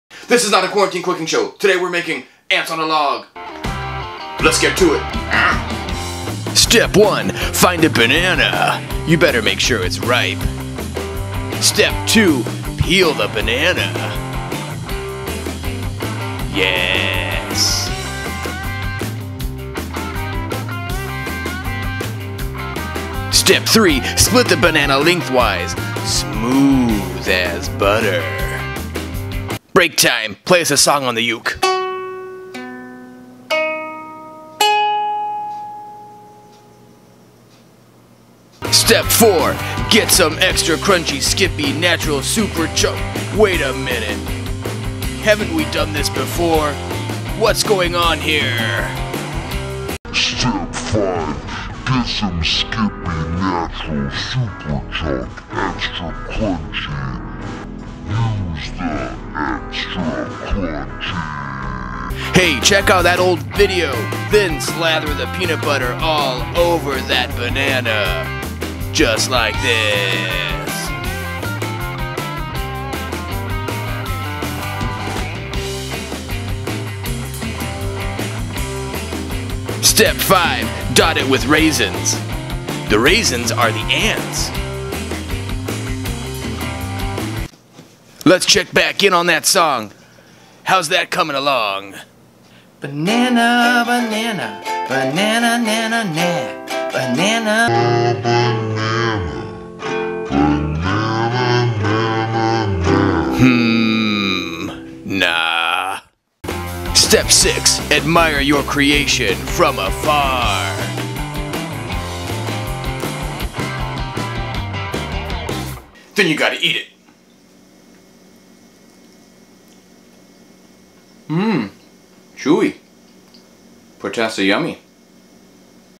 Quarantine Cooking! Ants on a Log!
Who needs celery? We've got bananas. We're making ants on a log. The raisins are the ants. Uh huh.